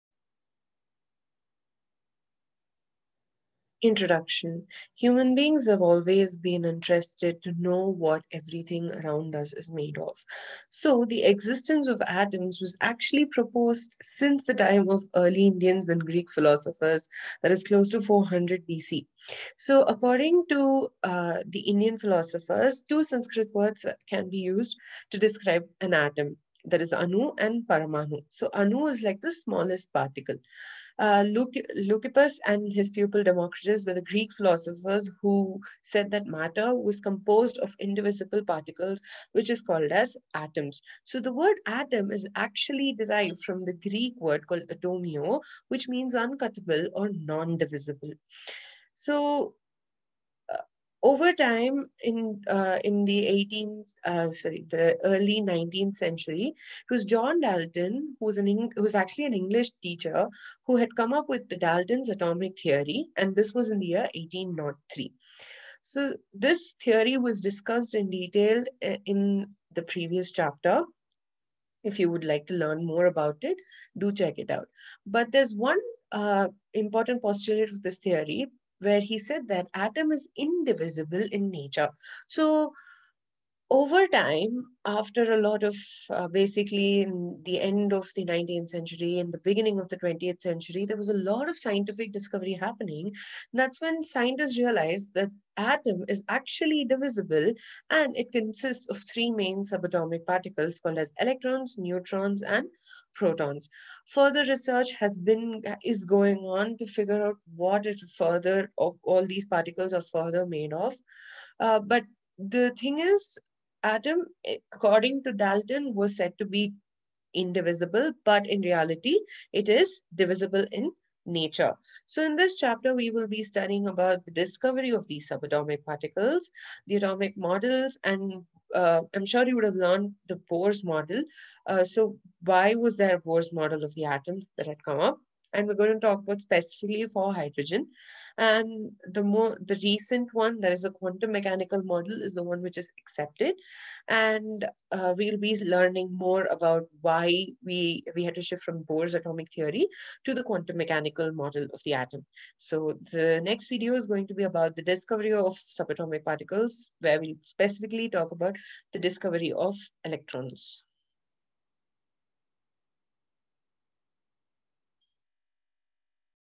Introduction to Structure of Atom
This video is the introduction to the second chapter for class 11 NCERT Chemistry.
The images have been obtained from the following sources

Democritus
Jan de Bisschop, CC0, via Wikimedia Commons 8/8f/Portretbuste_van_Democritus_Paradigmata_graphices_variorum_artificum_%28serietitel%29%2C_RP-P-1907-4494.jpg)

Check out the next video for the topic- Discovery of electron
If any doubts, do reach out in the comments section or by mail!